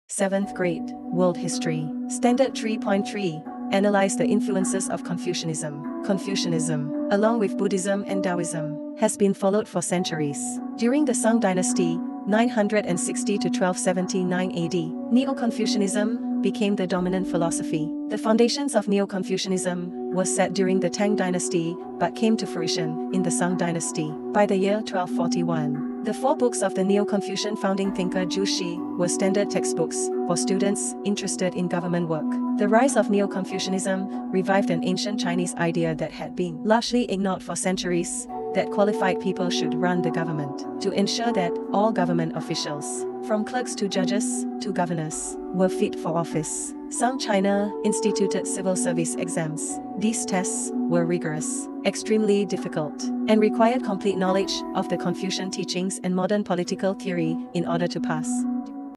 Confucianism fantasy landscape ai art buddhism confucianism china mongolia religion
7th Grade, World History. Standard 3.3: Analyze the influences of Confucianism. Confucianism, along with Buddhism and Daoism, has been followed for centuries. During the Sung Dynasty (960 to 1279 AD), Neo-Confucianism became the dominant philosophy. The foundations of Neo-Confucianism were set during the Tang Dynasty but came to fruition in the Sung Dynasty. By the year 1241, the Four Books of the Neo-Confucian founding thinker Zhu Xi were standard textbooks for students interested in government work. The rise of Neo-Confucianism revived an ancient Chinese idea that had been largely ignored for centuries: that qualified people should run the government. To ensure that all government officials, from clerks to judges to governors, were fit for office, Sung China instituted civil service exams. These tests were rigorous, extremely difficult, and required complete knowledge of the Confucian teachings and modern political theory in order to pass. During the Mongol period (1271 to 1368 AD), Confucianism changed as religion turned more political than religious.